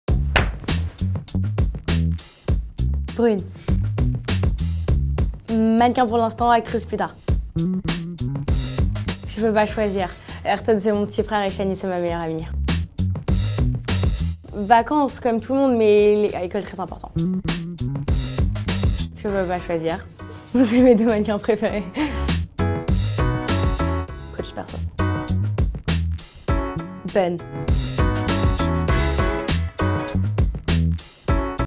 THIS OR THAT : THYLANE BLONDEAU PRÉFÈRE-T-ELLE GIGI OU BELLA HADID ? | AUFEMININ
On retrouve la jolie Thylane Blondeau pour une interview Tu préfères. À seulement 17 ans, Thylane est une mannequin très prisée !

Elle commence le mannequin à l'âge de 3 ans, en défilant pour Jean-Paul Gaultier à la Fashion Week de Paris. Elle est même surnommée, la plus belle enfant du monde !

Aujourd'hui Thylane Blondeau continue sa carrière de mannequinat en travaillant pour des marques comme Dolce & Gabbana, Vogue ou encore L'Oréal pour qui elle est égérie.

Thylane nous révèle qui elle préfère entre les top model Gigi et Bella Hadid mais aussi ce qu'elle préfère entre le mannequinat et le métier d'actrice vers lequel elle se dirige :)